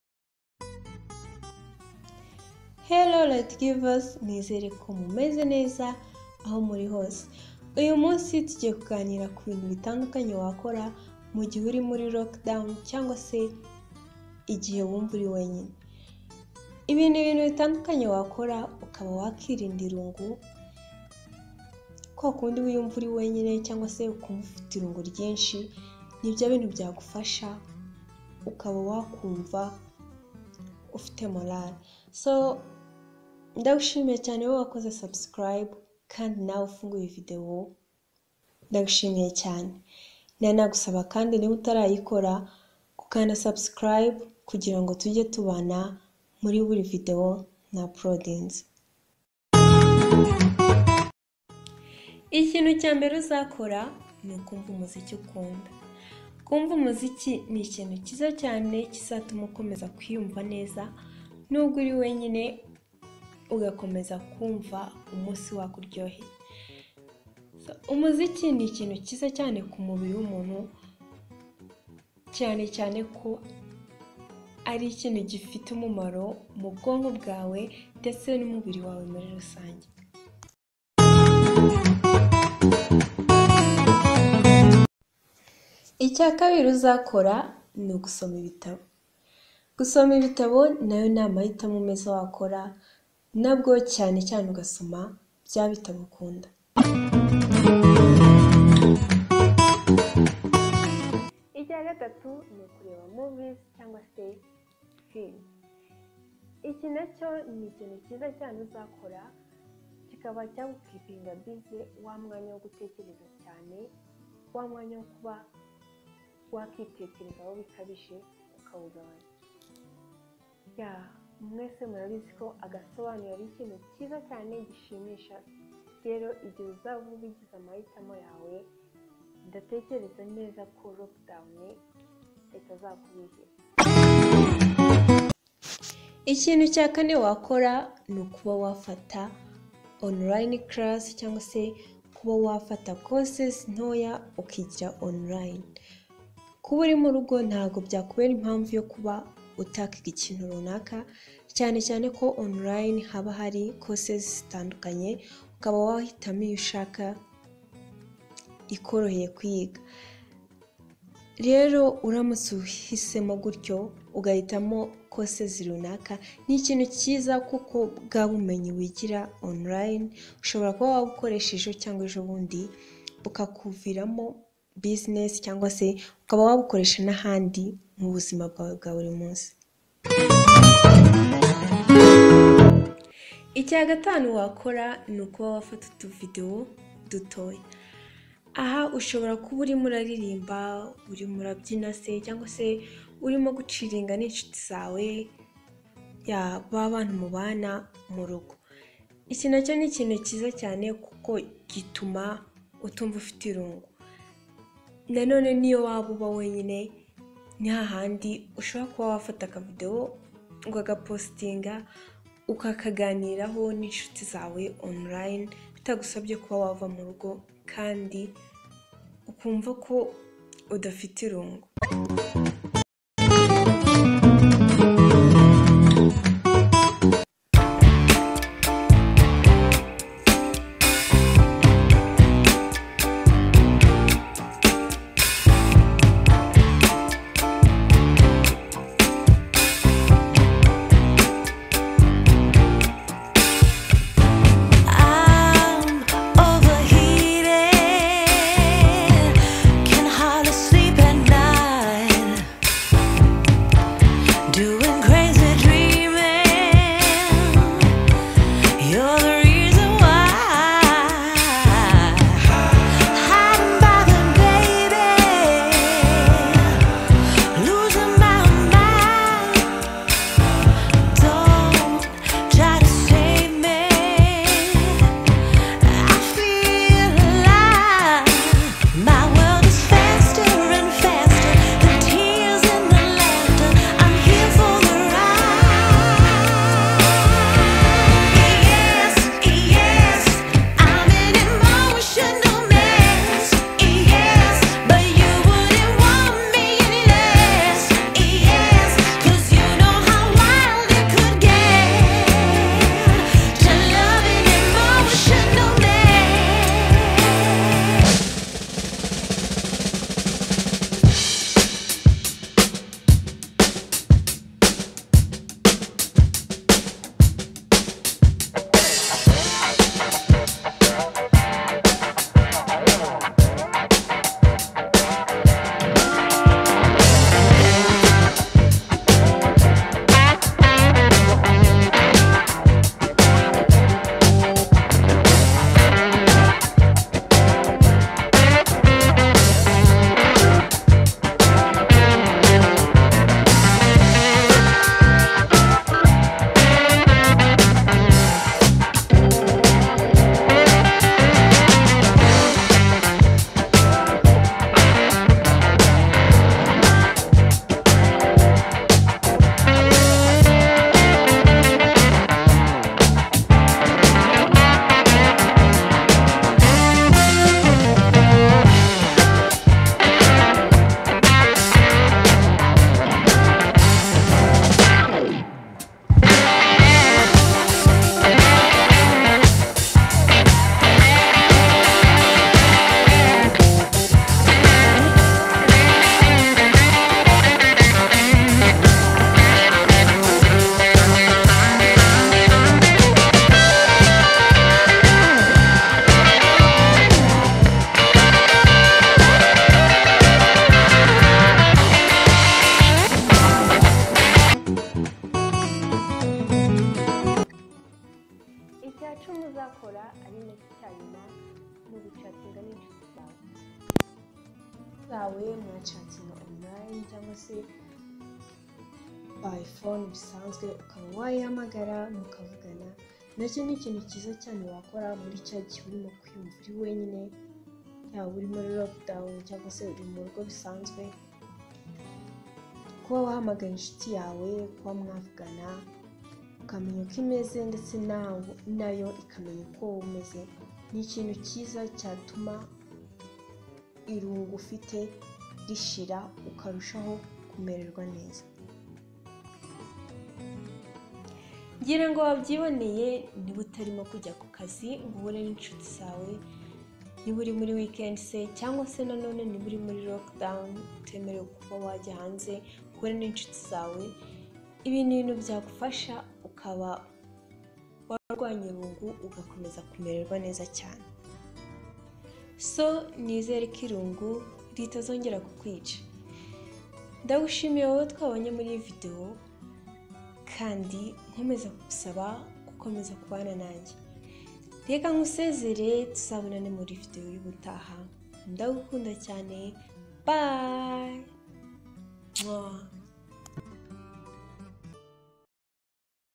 Dore uko warwanya irungu no gutekereza cyane mu gihe uri wenyine/ # Tips to overcome loneliness
Hello light givers! nizereko mumeze neza cyane!
Uyu munsi tugiye kuganira  ku  kuburyo  wahangana n'irungu mu gihe uri wenyine byumwihariko   muri lockdown.EnjoyThx!